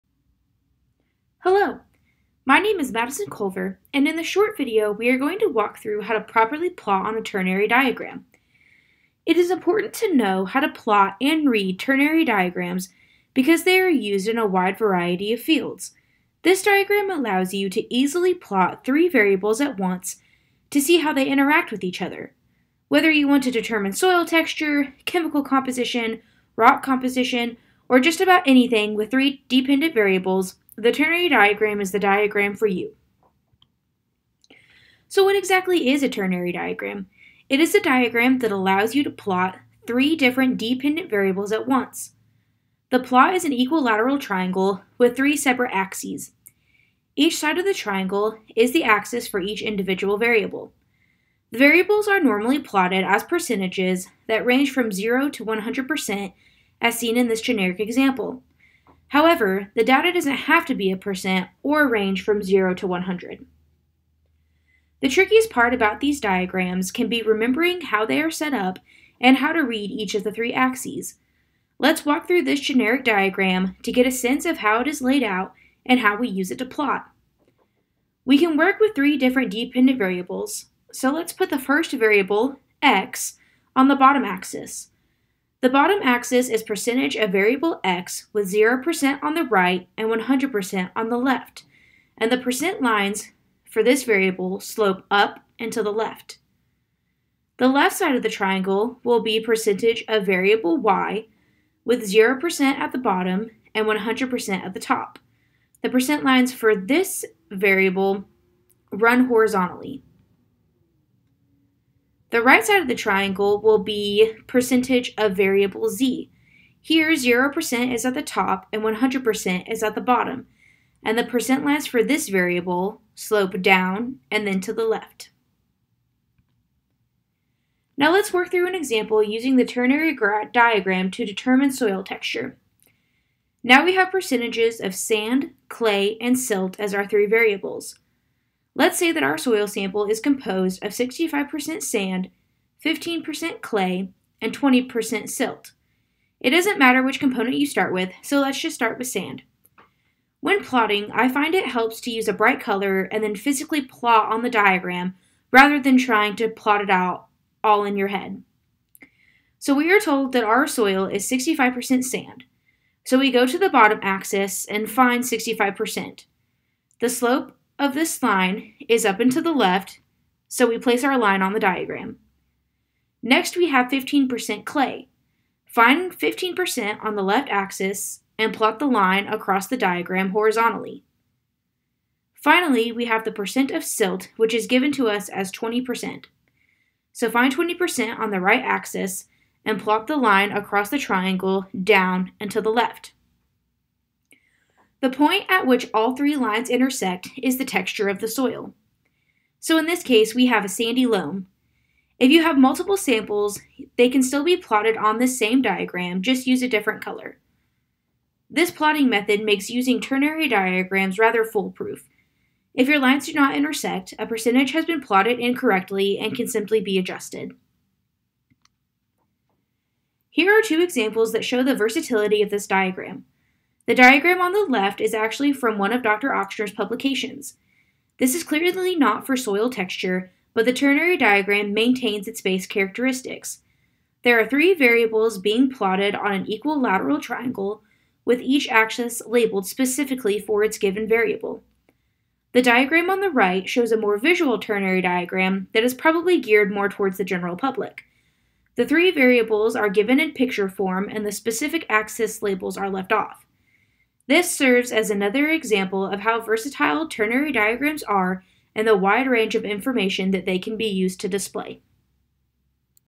Using a ternary diagram
This video explains how to read or plot data on a special triangle-shaped plot called a ternary diagram. Ternary diagrams are commonly used to represent the percent of sand, silt, and clay particles within a soil sample, for example. This video was created by Madison Culver in 2022, as part of the class "Soil, Water, and Weather" at Oklahoma State University. This video is used in the free e-textbook "Rain or Shine: An Introduction to Soil Physical Properties and Processes" by Tyson E. Ochsner, available